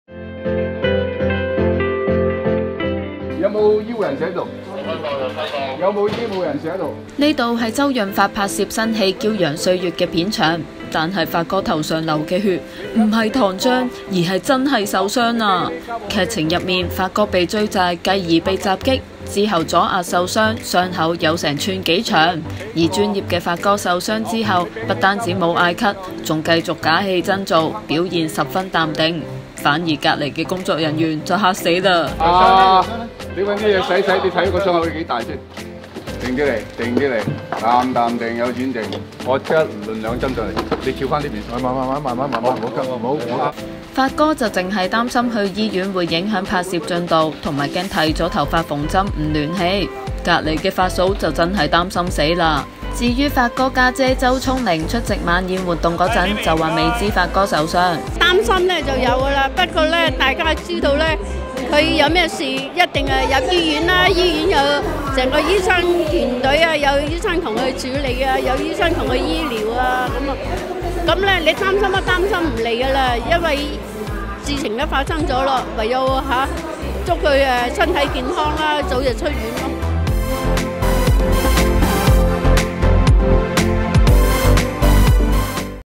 【娛樂】周潤發拍戲頭部受傷 胞姐周聰玲表示擔心 2019-03-08
周潤發日前為新片《驕陽歲月》拍攝一場追逐戲時受傷，頭部不幸被扑中當場血流如注，但意外發生時，發哥沒有即時叫停，繼續假戲真做，直至完成整場戲的演出！

血流披面的發哥隨即叫大家冷靜，又著人攞清水沖洗傷口，不停安慰大家「定啲、唔好驚！」發哥怕剃頭髮會不連戲，最後只剪了傷口附近最少範圍的頭髮，並縫了5針。

發哥姐姐周聰玲出席晚宴活動時，就表示未知發哥受傷，知道後也感到擔心，並祝他身體健康。

bmgproductionmusic